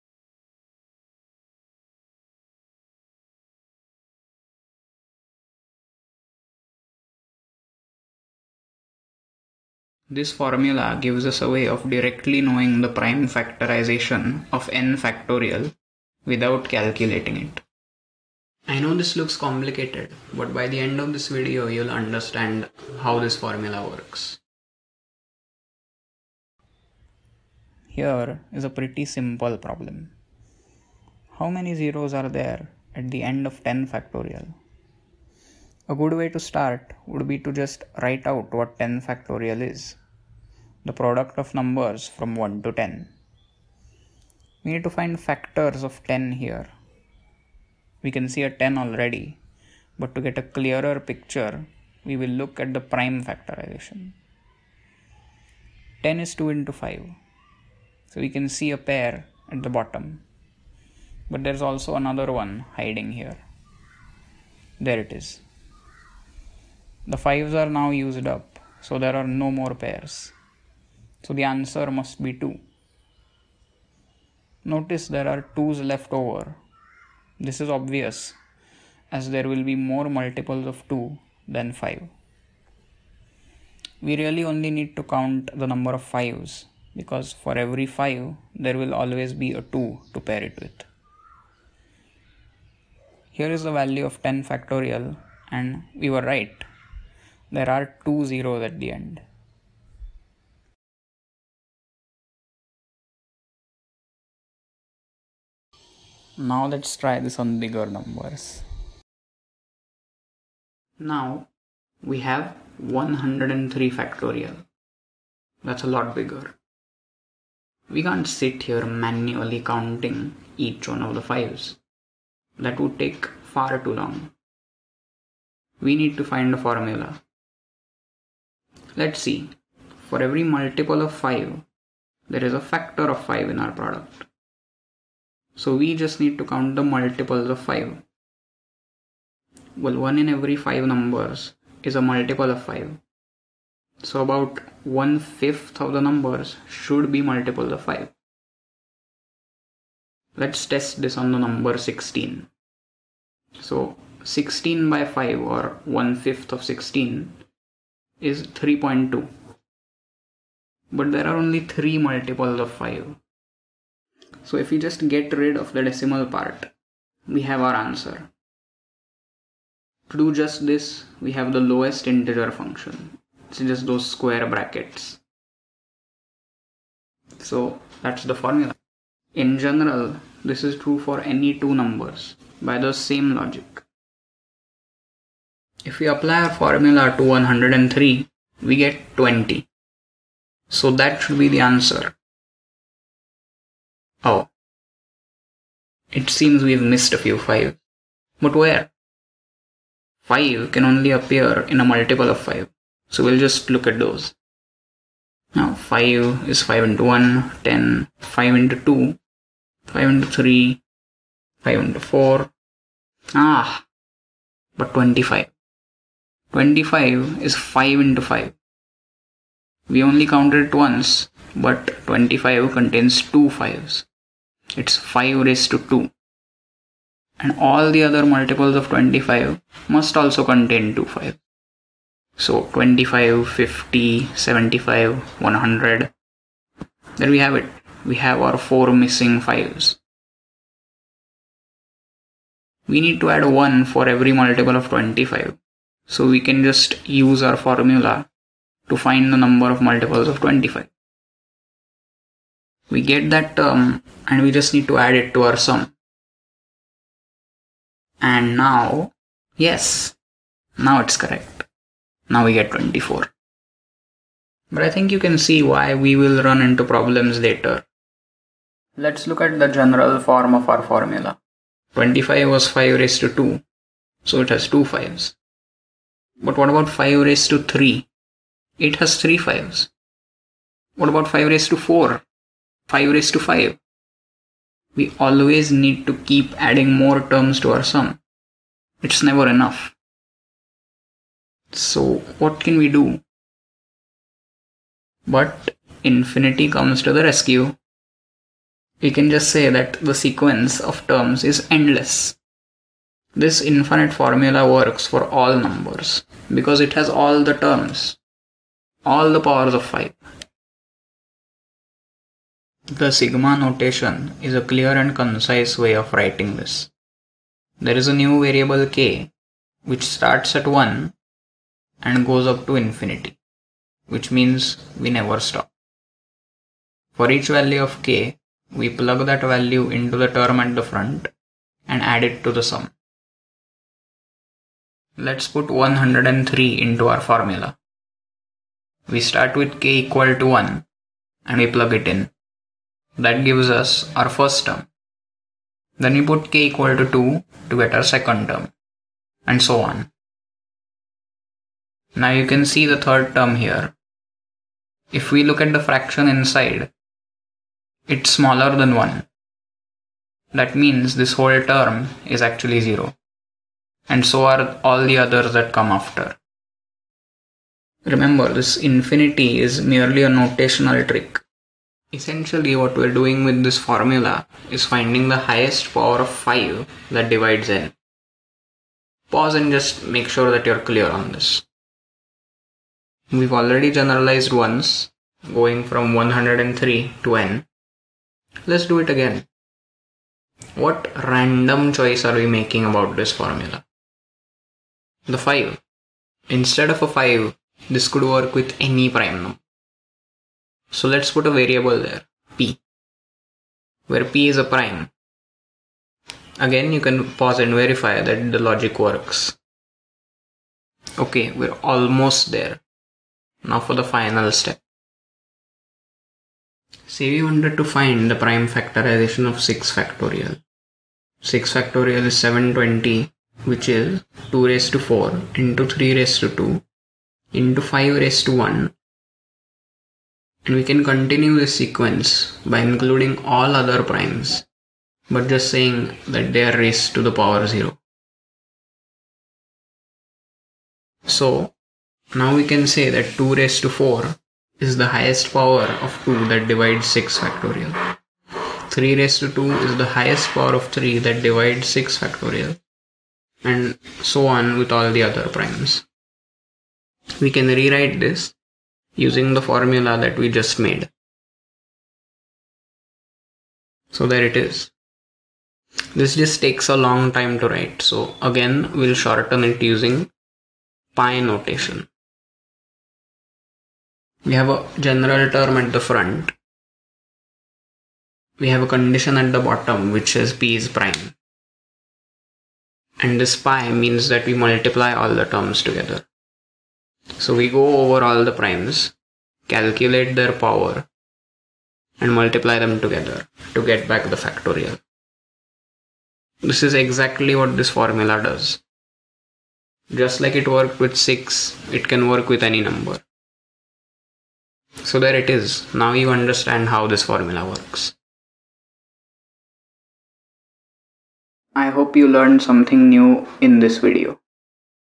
Legendre Formula Explained
In this video we see how the Legendre Formula works. How it can help us find the prime factorisation of factorial numbers